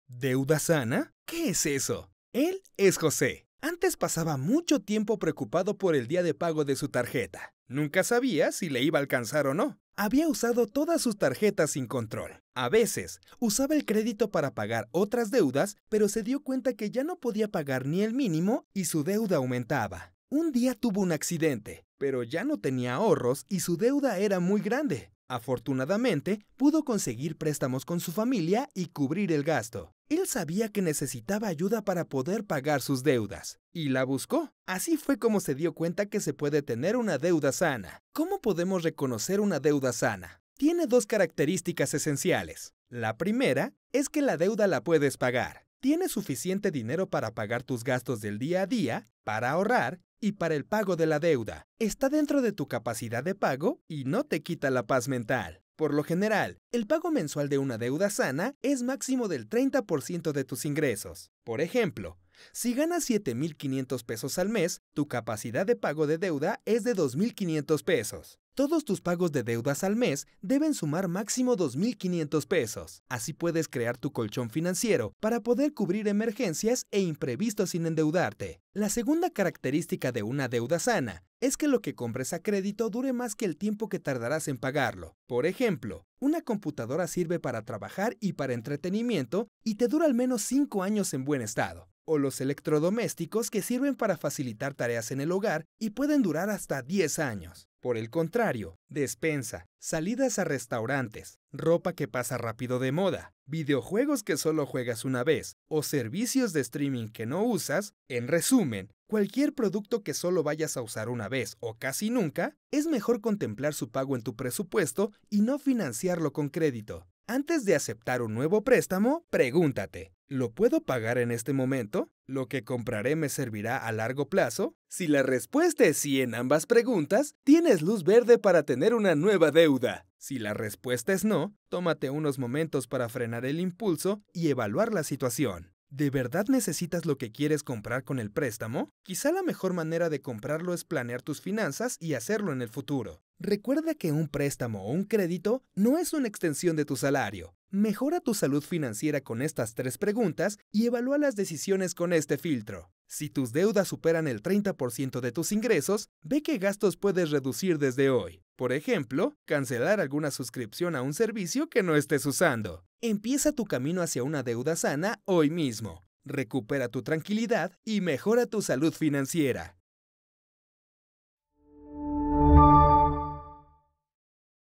¿Deuda sana? ¿Qué es eso?
Conocerás qué es una deuda sana y algunas acciones que puedes tomar para empezar a tener este tipo de deuda